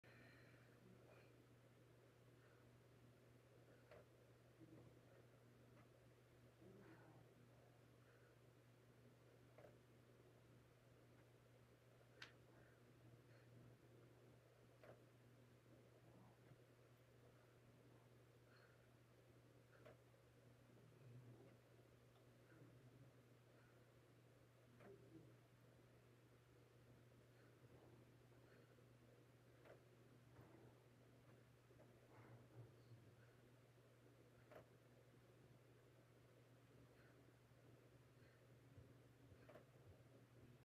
Reverse Lunge with Single-Arm Dumbbell Row
With a medium or heavy dumbbell in your left hand, lunge back with your right leg. Hold the lunge, placing your right hand on the chair for a bit of support, do one row, then press down through the right heel to come back to standing. Your back and lunging leg should move as one piece throughout the move. Do all reps on one side and then repeat on the other side.